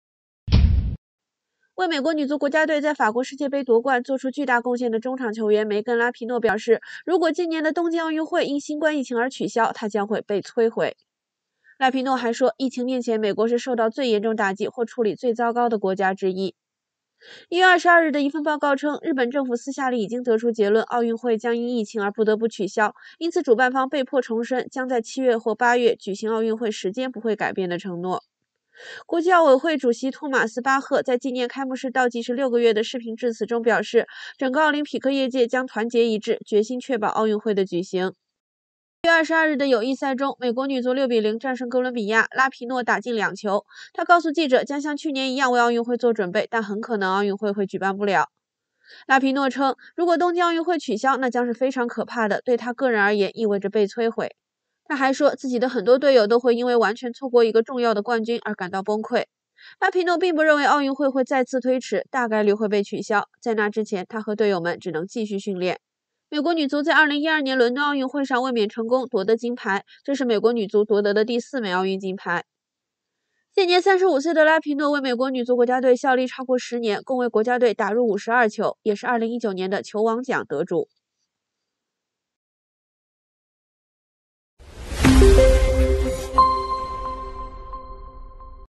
美国女足球星拉皮诺：若东京奥运会因疫情取消我会感到“被摧毁”
为美国女足在法国世界杯夺冠做出巨大贡献的中场球员梅根-拉皮诺表示，如果今年的东京奥运会因新冠疫情而取消，她将会 "被摧毁"。拉皮诺还说，疫情面前美国是受到最严重打击或处理最糟糕的国家之一。
1月22日的一份报告称，日本政府私下里已经得出结论，奥运会将因疫情而不得不取消，因此主办方被迫重申将在7月和8月举行奥运会时间不会改变的承诺。

国际奥委会(IOC)主席托马斯-巴赫(Thomas Bach)在纪念开幕式倒计时六个月的视频致辞中表示，整个奥林匹克业界将团结一致，决心确保奥运会的举行。

在1月22日的友谊赛中，美国女足6-0战胜哥伦比亚，拉皮诺打进两球。她告诉记者，将像去年一样为奥运会做准备，但很可能奥运会举办不了。拉皮诺称，如果东京奥运会取消，那将是非常可怕的，对她个人而言意味着“被摧毁”。她还说，自己的很多队友都会因为完全错过一个重要的冠军而感到崩溃。拉皮诺并不认为奥运会再次推迟，大概率会被取消。在那之前，她和队友们只能继续训练。

美国女足在2012年伦敦奥运会上卫冕成功夺得金牌，这是美国女足夺得的第4枚奥运金牌。
35岁的拉皮诺为美国女足效力超过10年，共为国家队打入52球，也是2019年的球王奖得主。